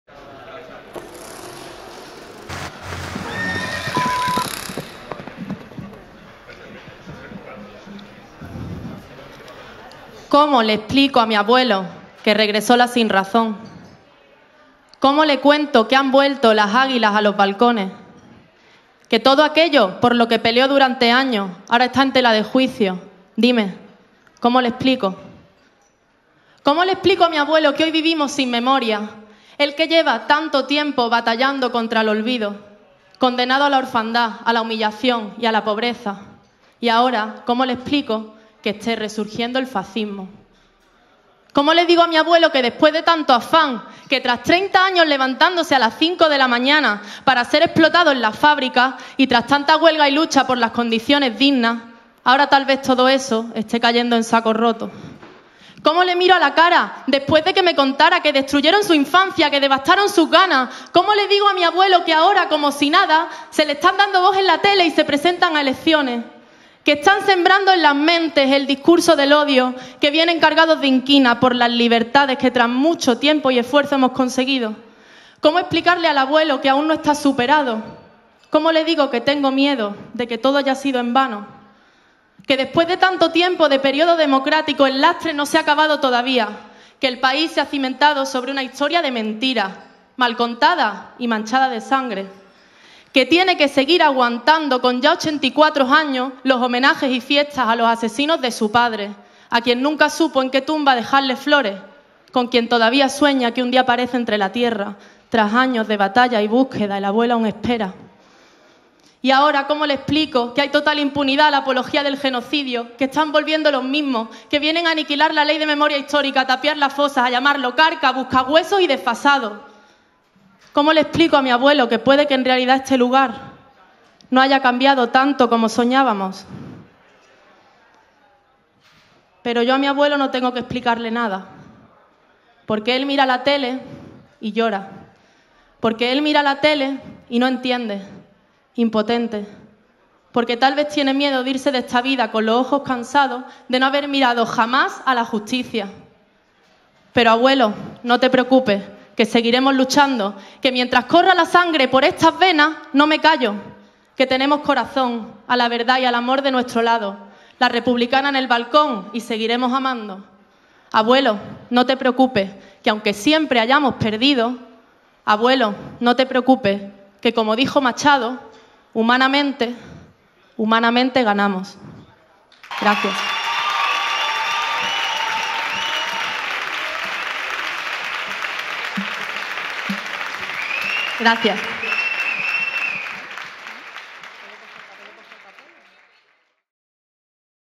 Memoria - Marina Gil
1ª Ronda de la Final de Poetry Slam Escaparate 2020.

Marina Gil representando a Poetry Slam Málaga. Recita: Memoria. Pasaría a la siguiente ronda.

Marina Gil: @marinalcuadrado
Escaparate de Poesía: @escaparatedepoesia